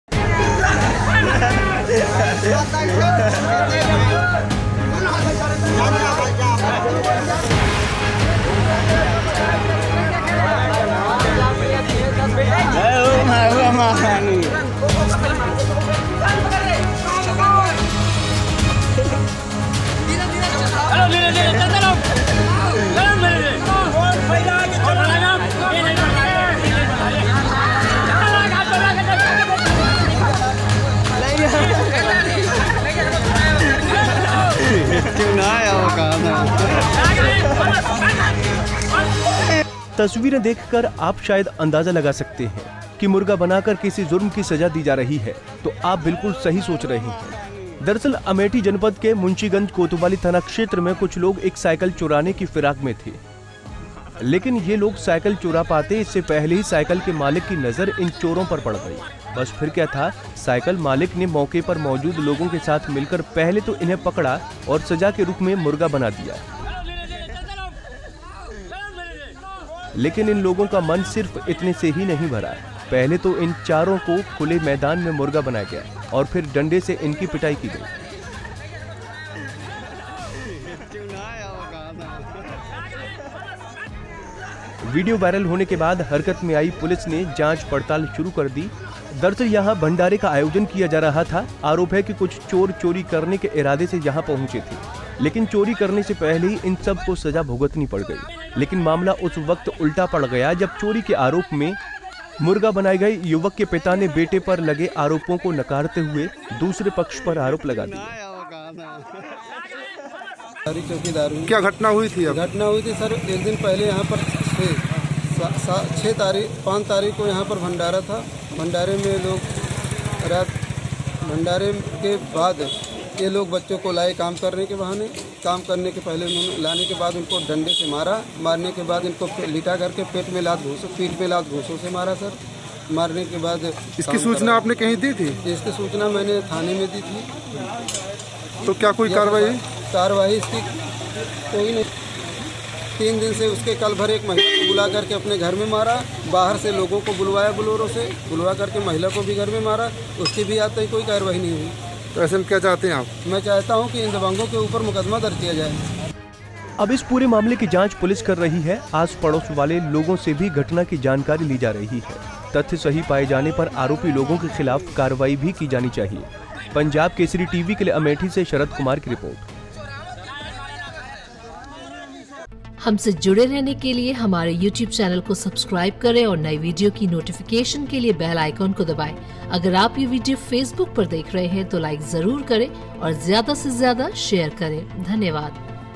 अमेठी: चोरी करने के आरोप में दी गई तालिबानी सजा, वीडियो भी वायरल कर दिया
तस्वीरें देख कर आप शायद अंदाजा लगा सकते है कि मुर्गा बनाकर किसी जुर्म की सजा दी जा रही है...तो आप बिल्कुल सही सोच रहे है...दरअसल अमेठी जनपद के मुंशीगंज कोतवाली थाना क्षेत्र में कुछ लोग एक साइकिल चुराने की फिराक में थे...लेकिन ये लोग साइकिल चुरा पाते इससे पहले ही साइकिल के मालिक की नजर इस चोरों पर पड़ गई...बस फिर क्य़ा था साइकिल मालिक ने मौके पर मौजूद लोगों के साथ मिलकर पहले तो इन्हे पकड़ा और सजा के रुप में मुर्गा बना दिया...लेकिन इन लोगों का मन सिर्फ इतने से ही नहीं भरा...पहले तो इन चोरों को खुले मैदान में मुर्गा बनाया गया...और फिर डंडे से इनकी पिटाई की गई...वीडियो वायरल होने के बाद हरकत में आई पुलिस ने जांच पड़ताल शुरु कर दी...दरअसल यहां भंडारे का आयोजन किया जा रहा था आरोप है कि कुछ चोर चोरी करने के इरादे से यहां पहुंचे थे. लेकिन चोरी करने से पहले ही इन सब को सजा भुगतनी पड़ गई...लेकिन मामला उस वक्त उल्टा पड़ गया जब चोरी के आरोप में मुर्गा बनाए गए युवक के पिता ने बेटे पर लगे आरोपों को नकारते हुए दूसरे पक्ष पर आरोप लगा दिए...